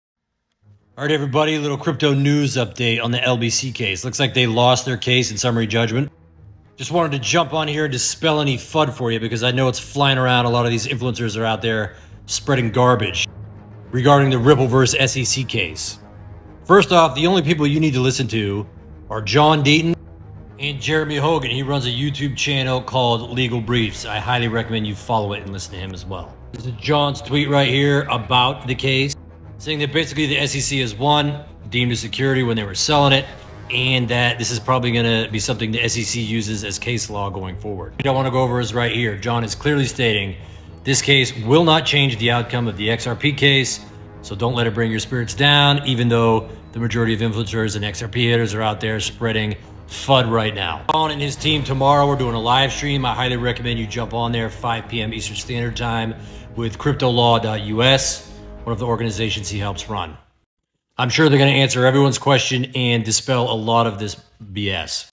🚨XRP Alert 🚨 LBC loses case against SEC. This will not affect Ripple vs SEC
One of the biggest mistakes you can make it investing is listening to other people spreading fud. ￼ They are out there to manipulate you and make you sell your investment. Don’t do it listen to the people in the know.